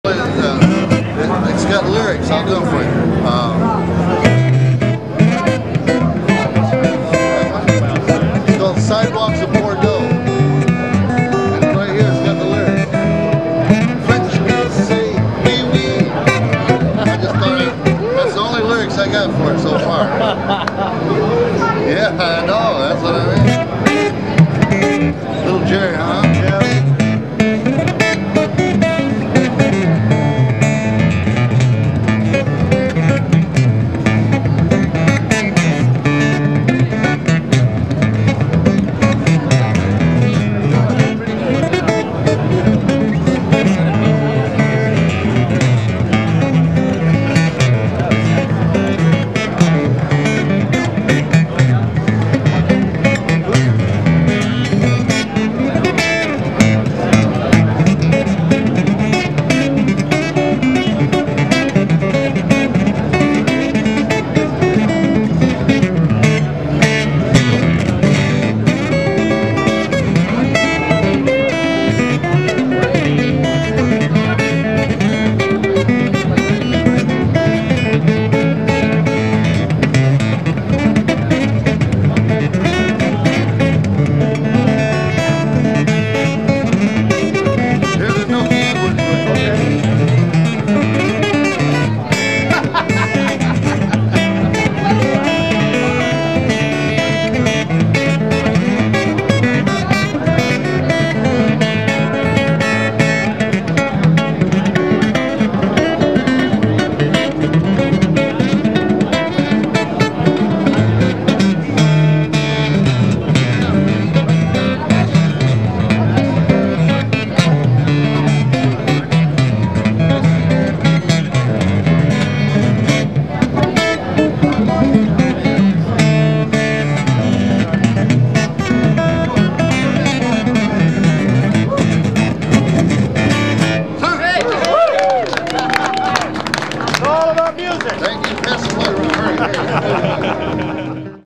Thom Bresh plays Jerry reed on a Stonebridge Guitar
This is a clip of Thom Bresh playing a Jerry Reed tune on a Stonebridge 23CR at winter NAMM 2007.